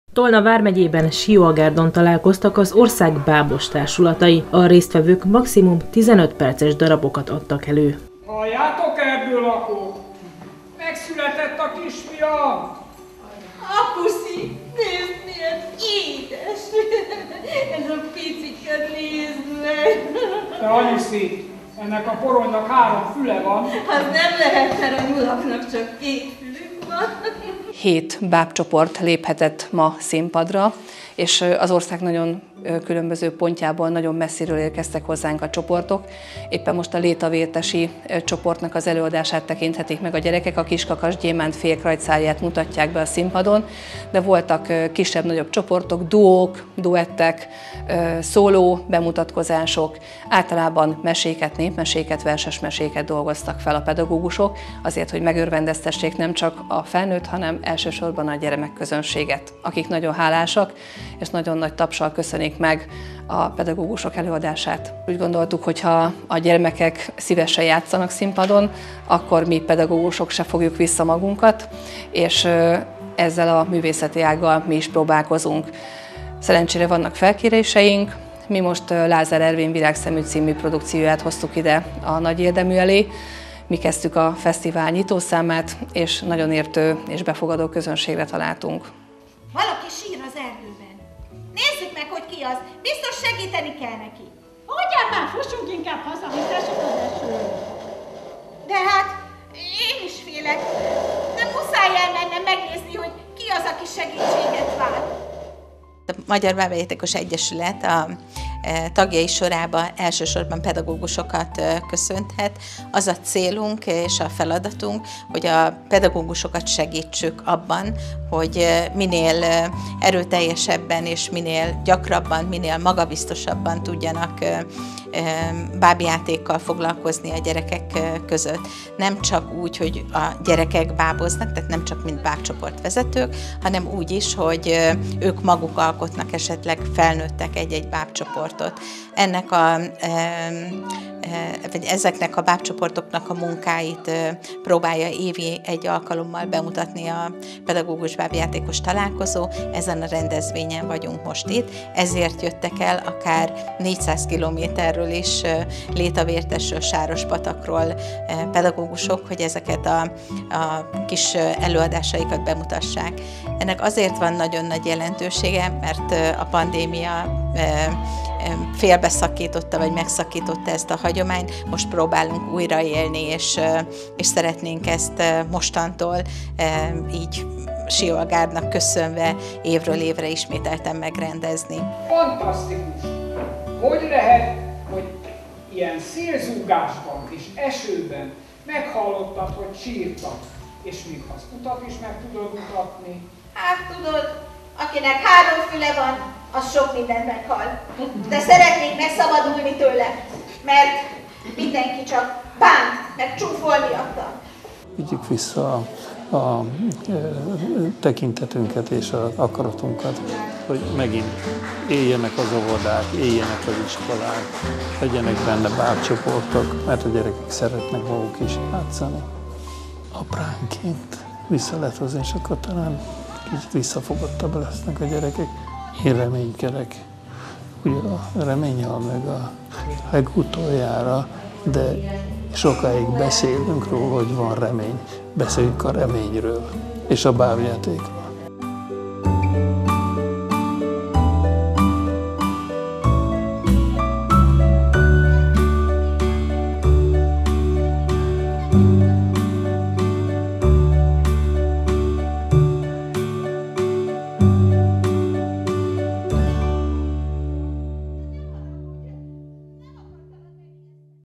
Tolna vármegyében találkoztak az ország bábos társulatai
Tolna vármegyében, Sióagárdon találkoztak az ország bábos társulatai. A rendezvény célja az volt, hogy a résztvevők megmutassák, ez a művészeti ág nem csak gyerekeknek, felnőtteknek is tartogat szórakoztatási lehetőségeket.